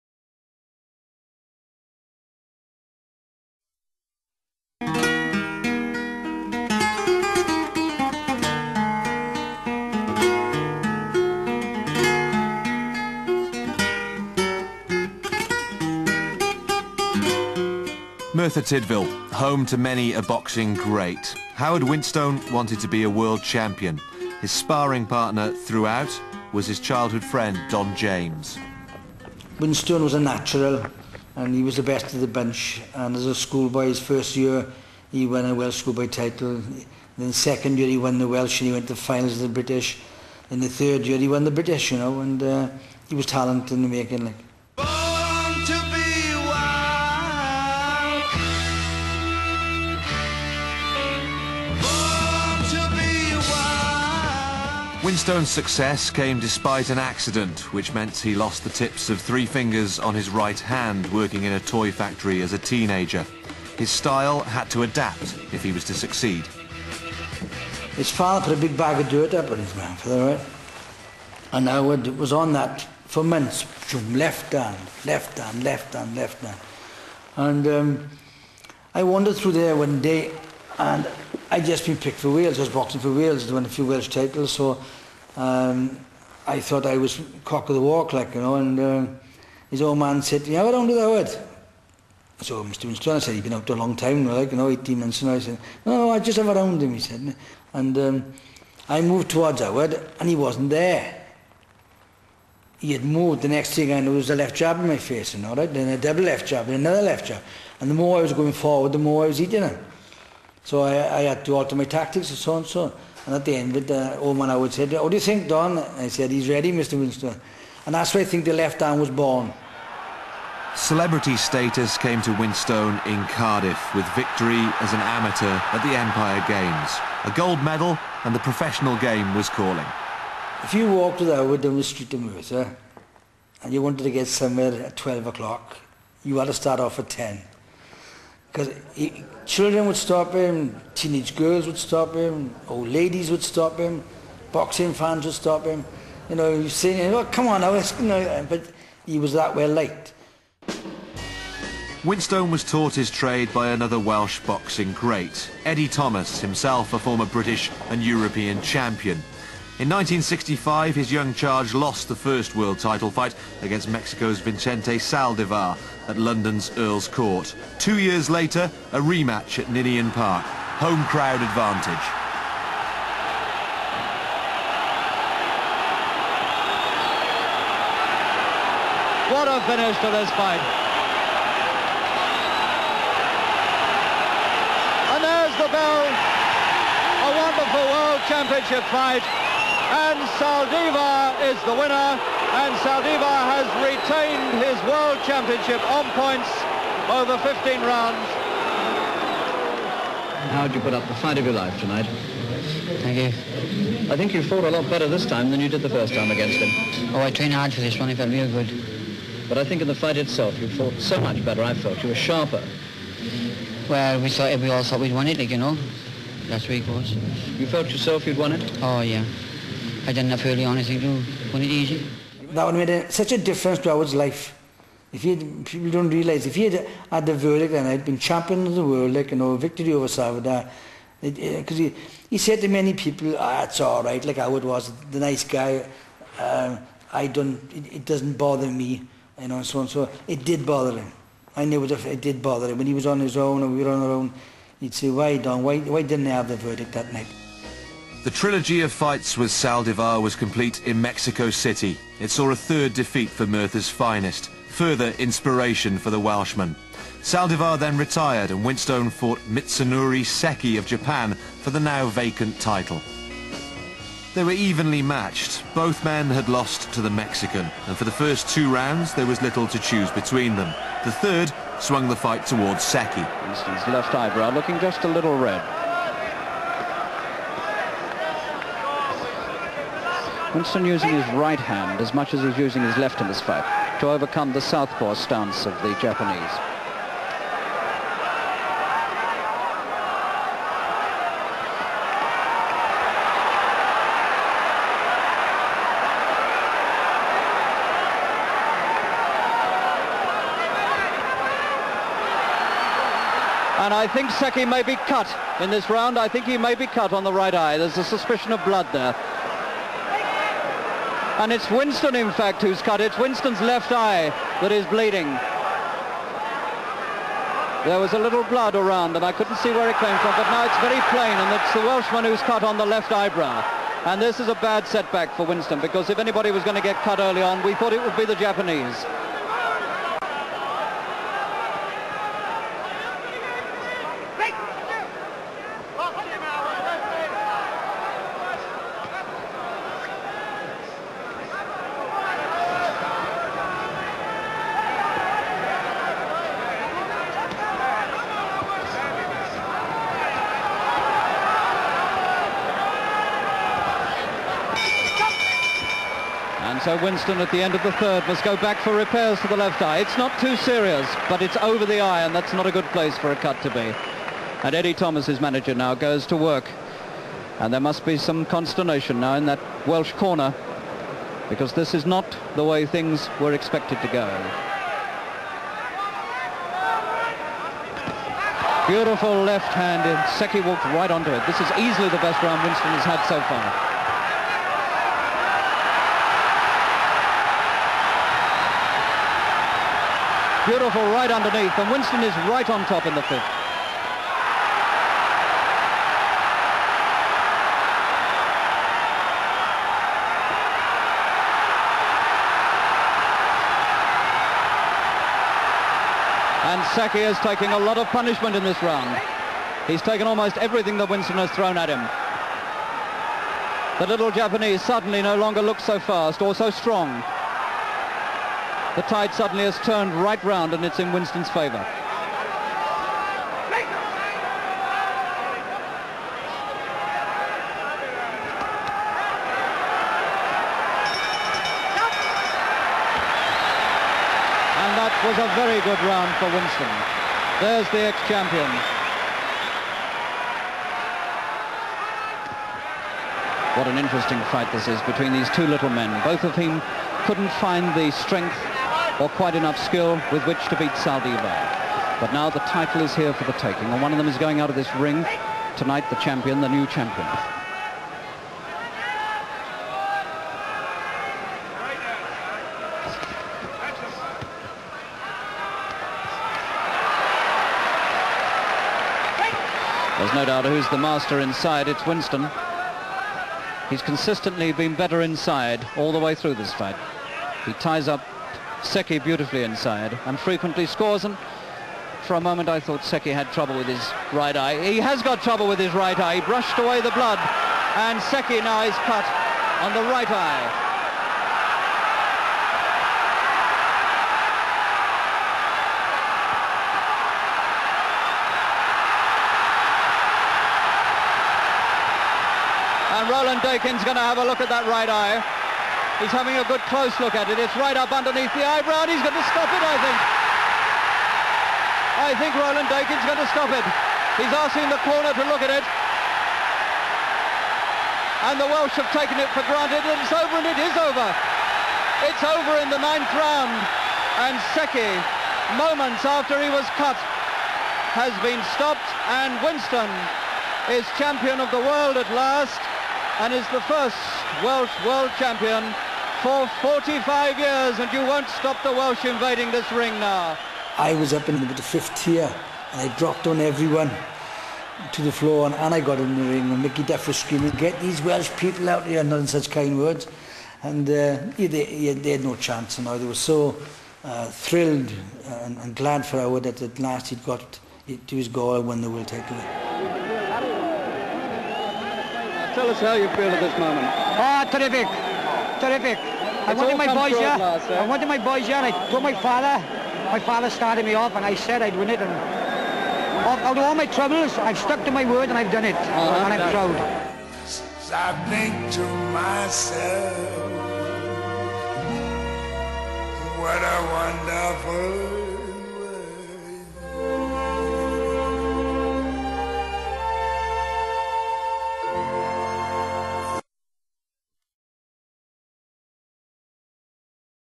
Howard Winston Tribute
A look back at Howard Winston attempts at winning the World Featherweight Title